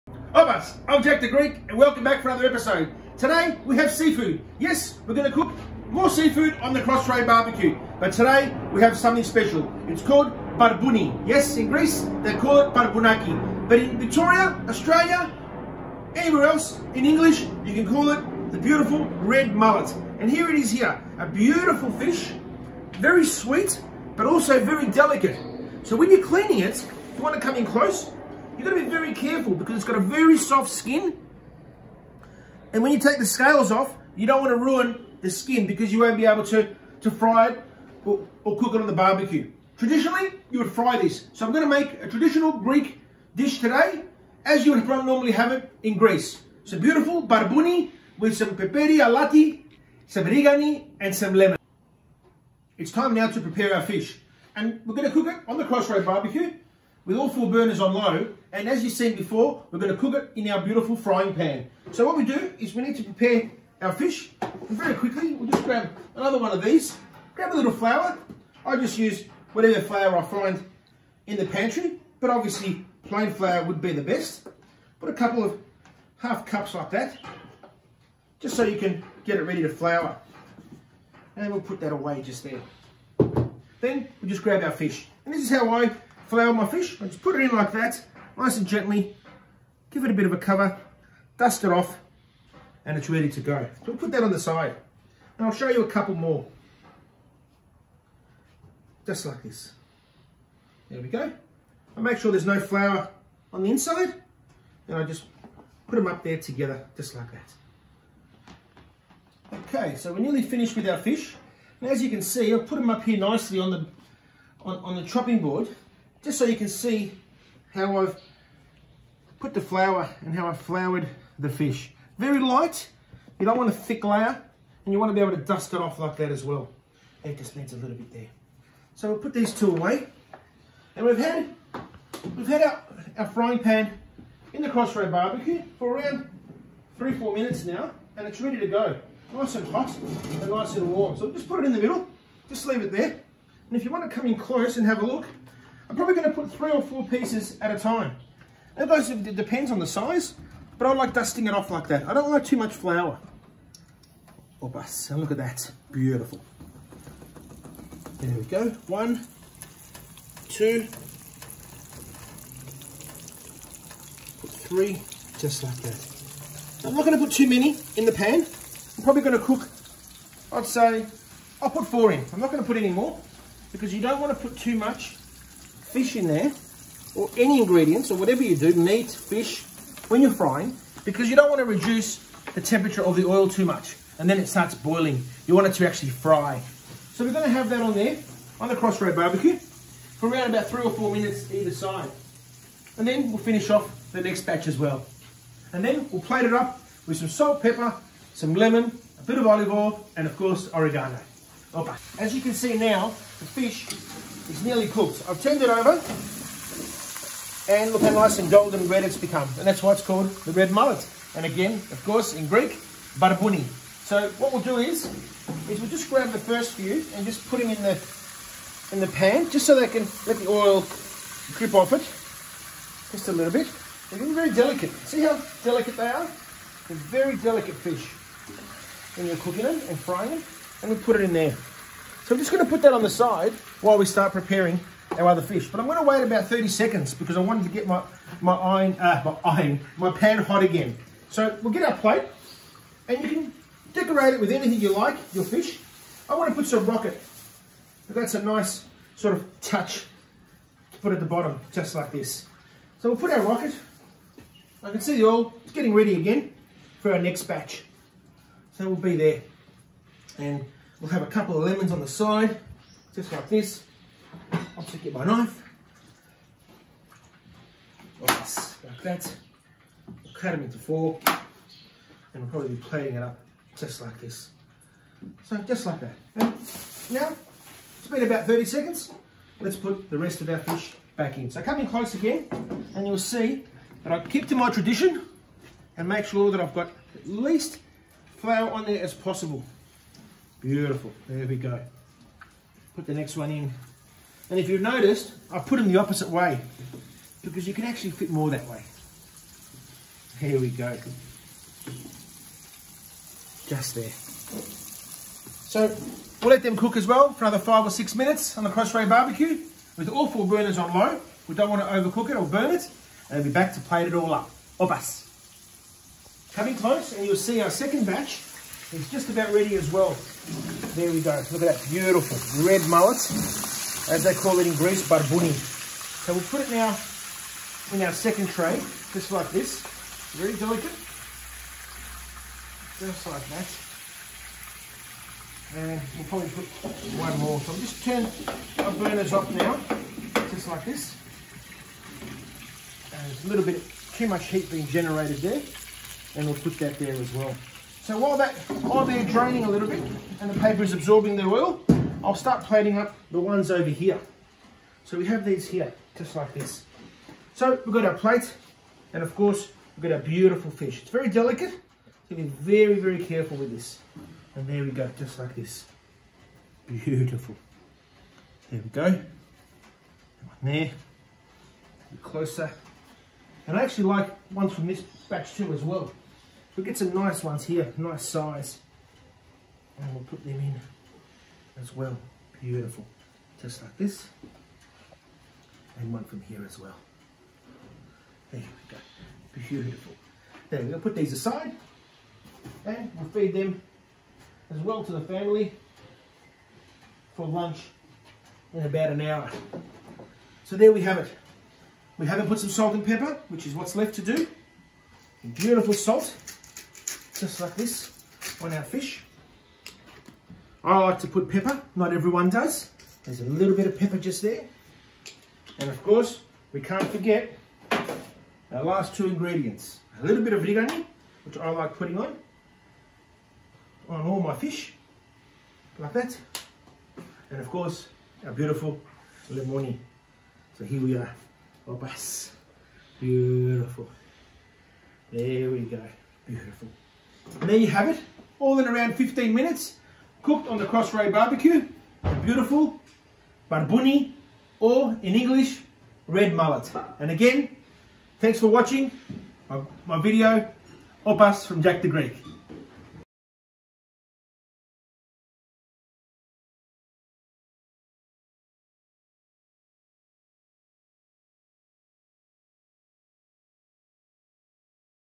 Greek Style Red Mullet (Barbouni) by Jack The Greek
Once considered a delicacy by the ancient Romans, barbouni is a type of goatfish native to the Mediterranean Sea. Today, the small, pinkish fish is a staple in Greek fish taverns, prized for its mildly sweet flavour.

Today we cooked it on the CrossRay BBQ, demonstrating how easy it is to fry fish outdoors using the heatstrip technology.

Keeping it simple - Lightly floured and topped with some fresh lemon - that's all you need to enjoy any beautiful fresh fish whether it is grilled or fried.